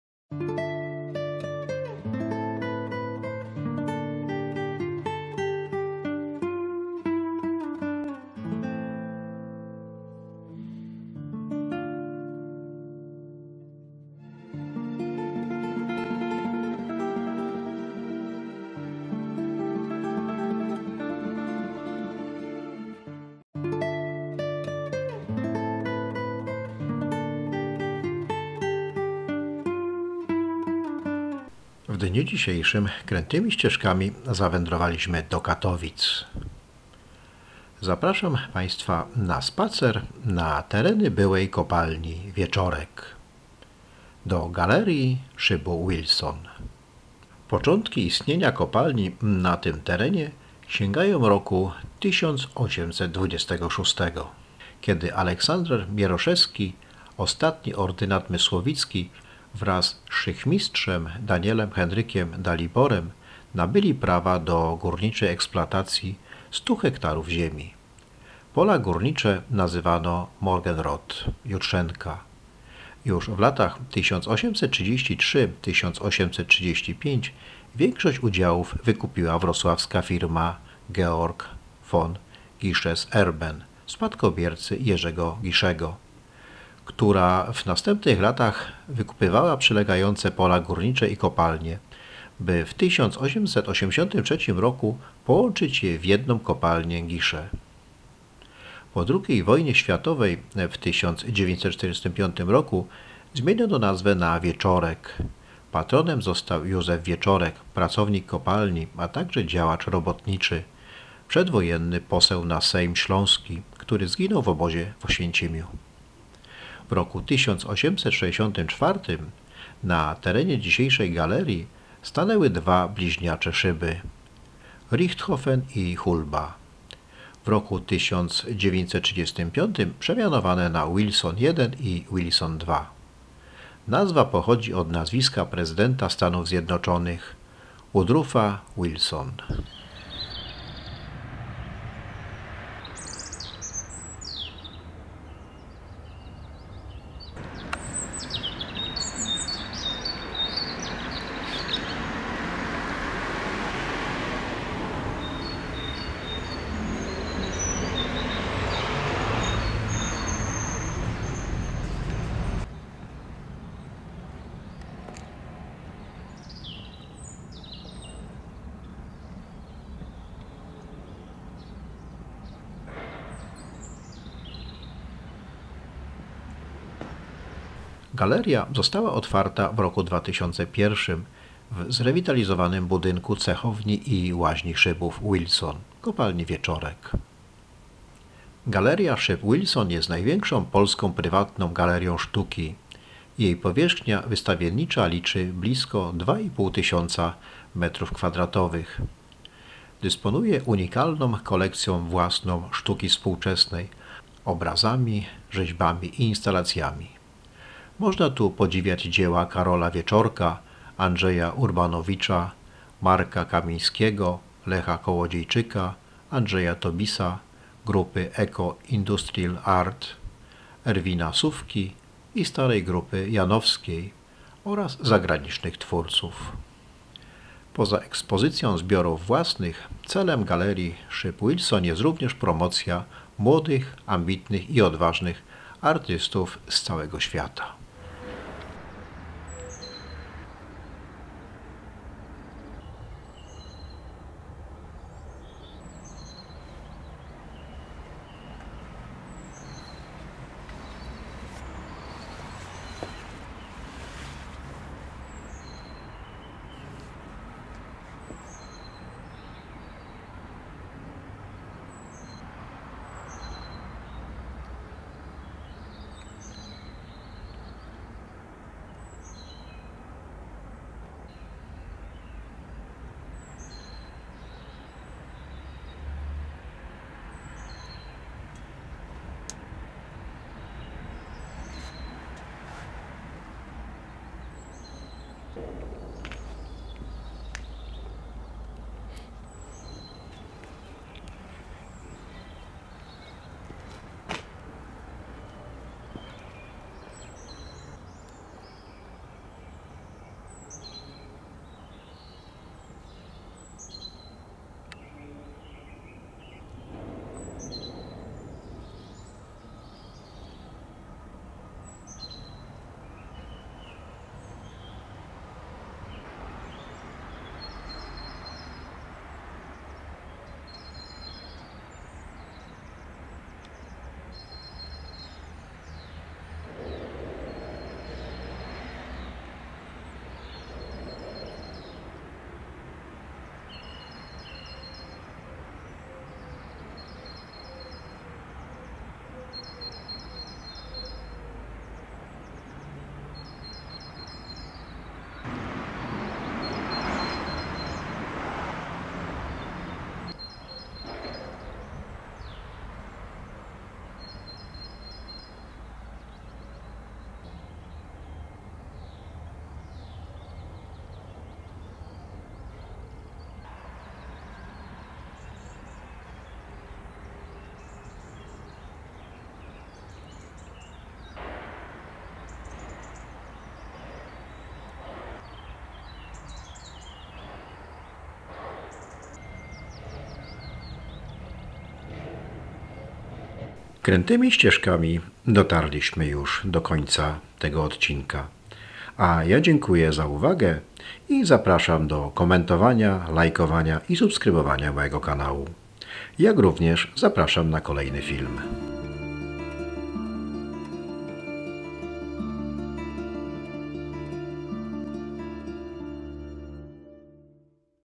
Galeria. Szyb Wilson w Katowicach. KWK Wieczorek. Kopalnia „Giesche”. Śląskie. Polska. Poland.
Twórcami galerii są Monika Paca-Bros i Johann Bros. Głównym celem galerii obok rewitalizacji społecznej dzielnicy jest promocja młodych, zaangażowanych twórców oraz prezentacja polskiej i światowej sztuki współczesnej. W galerii oprócz wystaw sztuki, festiwali odbywają się koncerty, spektakle teatralne, warsztaty artystyczne i konferencje. Jest miejscem spotkań i poszerzania dyskursu publicznego.
Galeria Szyb Wilson jest największą polską prywatną galerią sztuki, jej powierzchnia wystawiennicza liczy blisko 2500 m².
Początki istnienia kopalni na tym terenie sięgają roku 1826, kiedy Aleksander Mieroszewski, ostatni ordynat mysłowicki, wraz z szychtmistrzem Danielem Henrykiem Daliborem nabyli prawa do górniczej eksploatacji 100ha ziemi. Pole górnicze nazwano „Morgenroth” (Jutrzenka). Już w latach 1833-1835 większość udziałów wykupiła wrocławska firma Georg von Giesches Erben – Spadkobiercy Jerzego Gieschego, która w następnych latach wykupywała przylegające pola górnicze i kopalnie, by w 1883 roku połączyć je w jedną kopalnię „Giesche”.